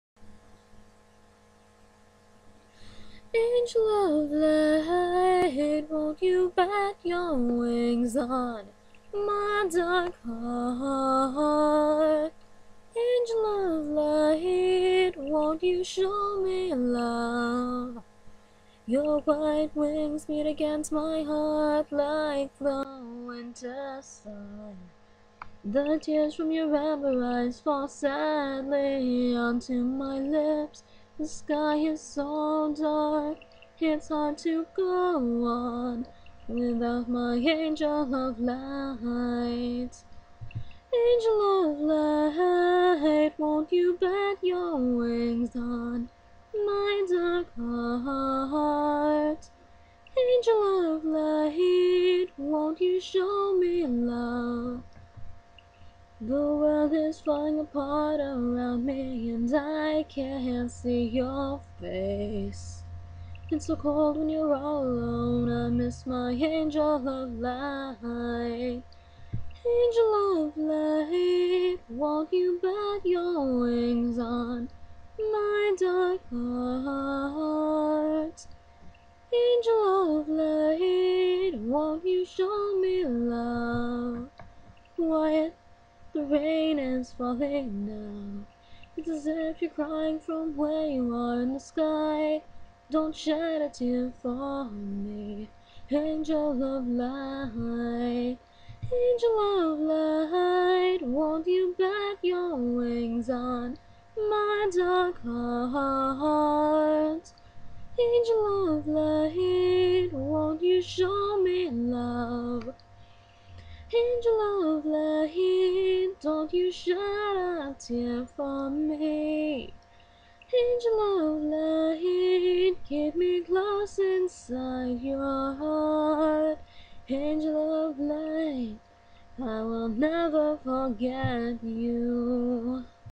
Angel of Light.wmv
Song of mine that I wrote, please excuse the electric drone, I recorded this with my computer.